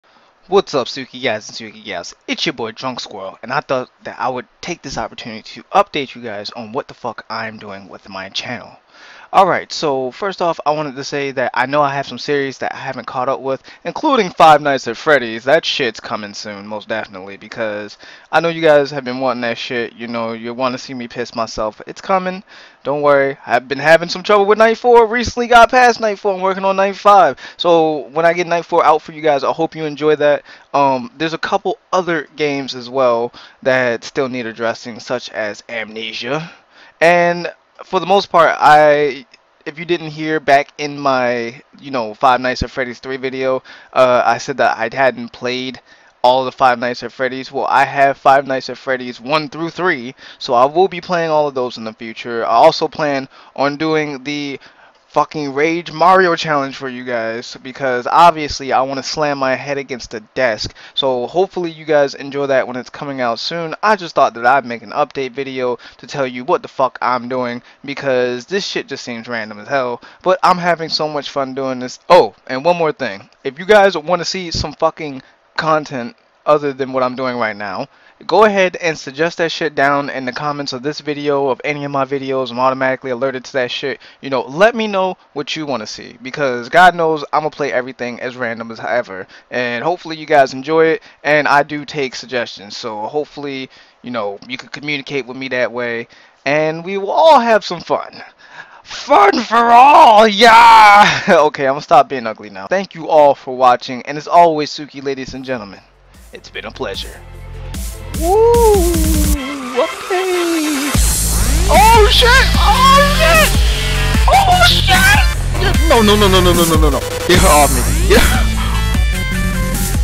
Drunk'Squirrel VLOGs - CHANNEL UPDATE
Wassup Tsuki-Guys & Tsuki-Gals, this short videya will explain whats to come in the soon future as Squirrel rambles for a minute or two :)